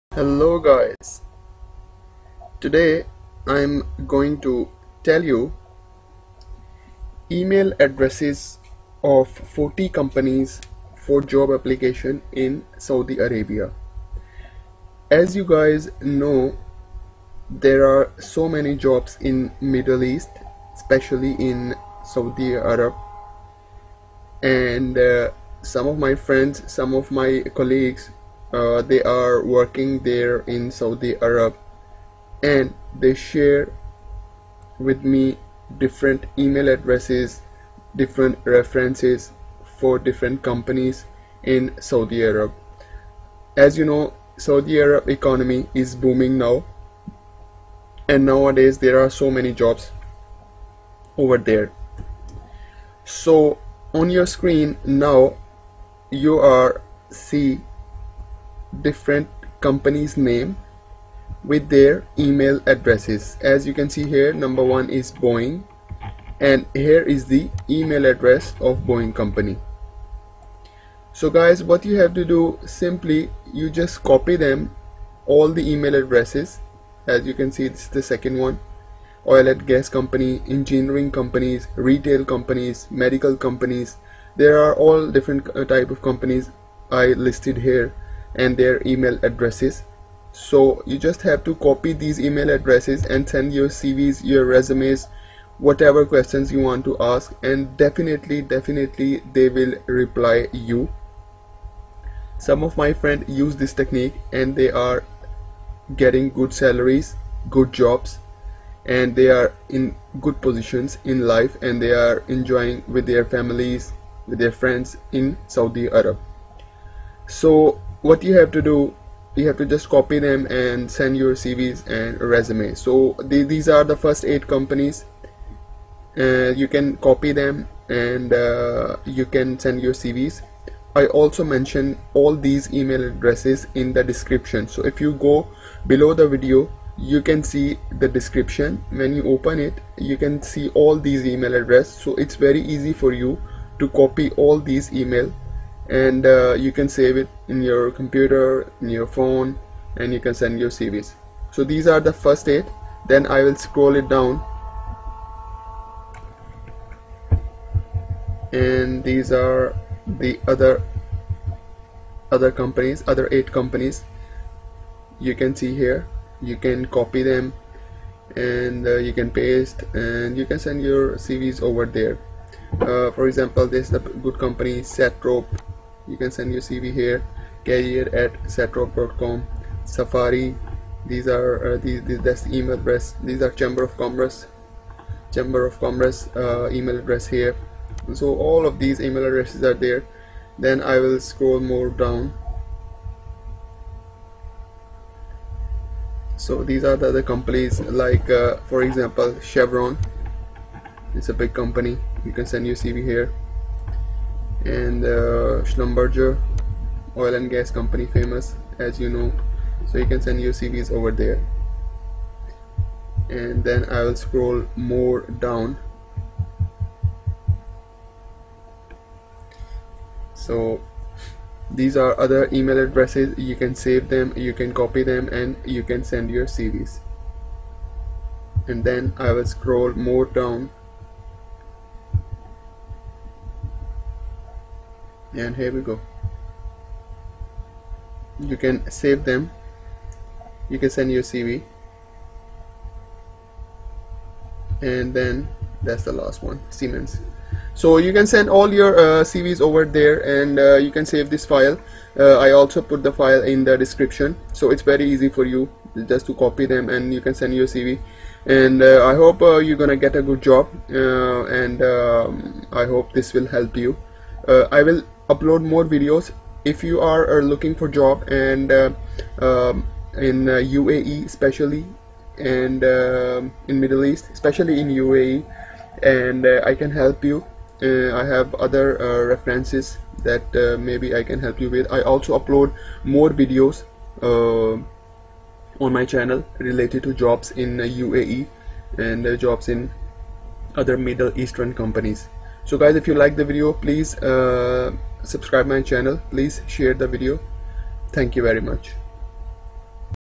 Jobs in Saudi Arab by email addresses
Jobs in Saudi Arab

Below are the list of companies in Saudi Arab
Email addresses of 40 companies for job application in Saudi Arabia.

16- Arabian Geophysical & Surveying Co

35- The Saudi Investment Bank